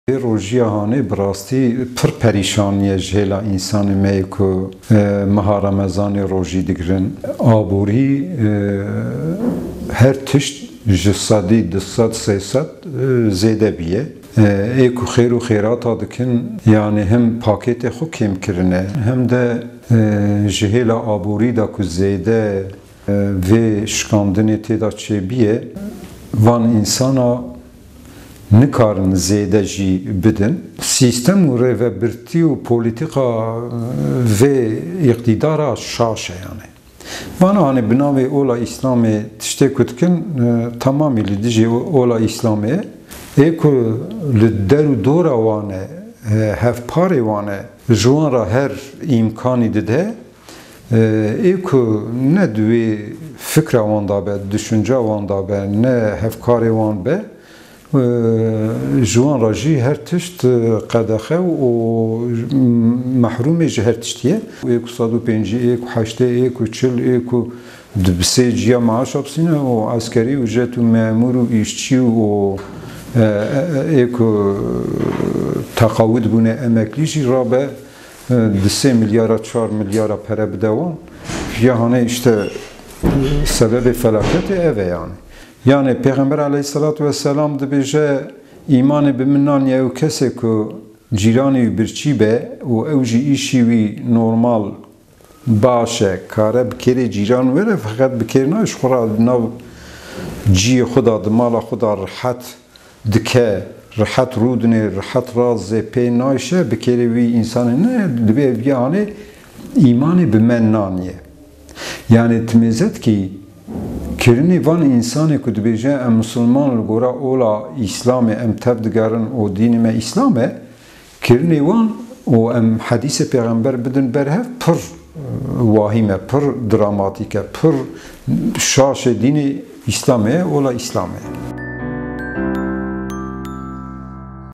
MELE HECÎ KADAH: MISILMANIYA HIKÛMETÊ TENÊ BI DEVKÎ YE
Endamê Komeleya Alîkarî û Pişevaniyê ya Dîndaran (DÎAY-DER) a bi KHK’ê hatibû girtin Mele Hecî Kadah bi wesîleya meha Remezanê bal kişand ser polîtîkayên AKP’ê û got: Misilmantiya hikûmetê tenê bi devkî ye.